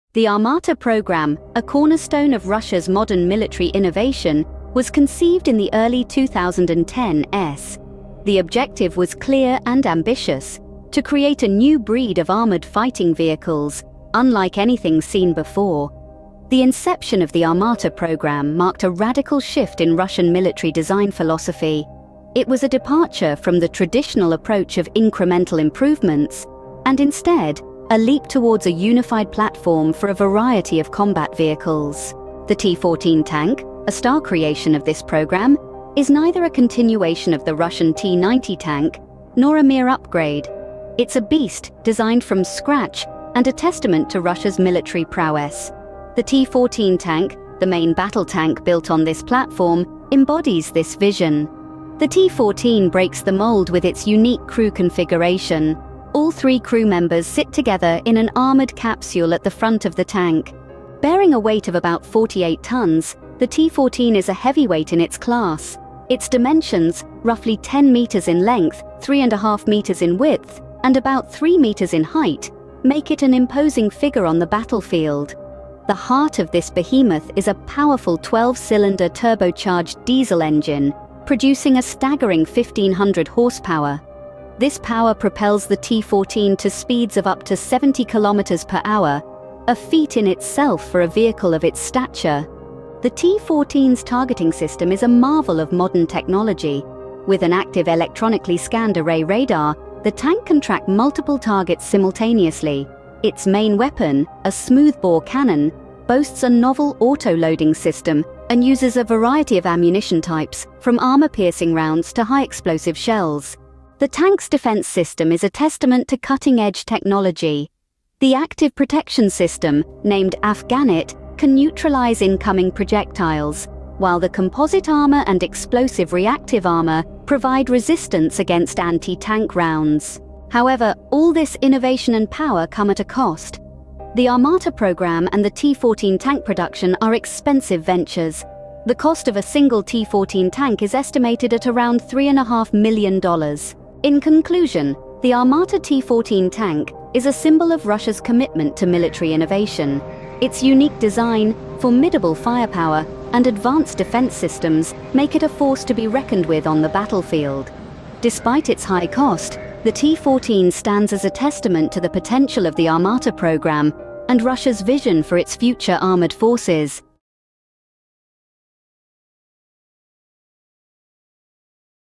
Armata T-14: Russia's Tank Revolution
Welcome to our latest video where we delve deep into the world of military technology and explore Russia's groundbreaking Armata T-14 tank! Join us as we uncover the design, specifications, and cutting-edge technology that make the Armata T-14 a revolutionary force on the battlefield. Don't miss out on this fascinating journey into the world of modern warfare.

OUTLINE:

00:00:00 The Birth of the Armata Program
00:00:32 The T-14 Tank's Unique Design
00:01:00 The Power and Speed of the T-14
00:01:32 Advanced Weaponry and Targeting System
00:01:53 Cutting-Edge Defense Systems
00:02:10 The Cost of Innovation
00:02:23 The T-14's Impact on the Battlefield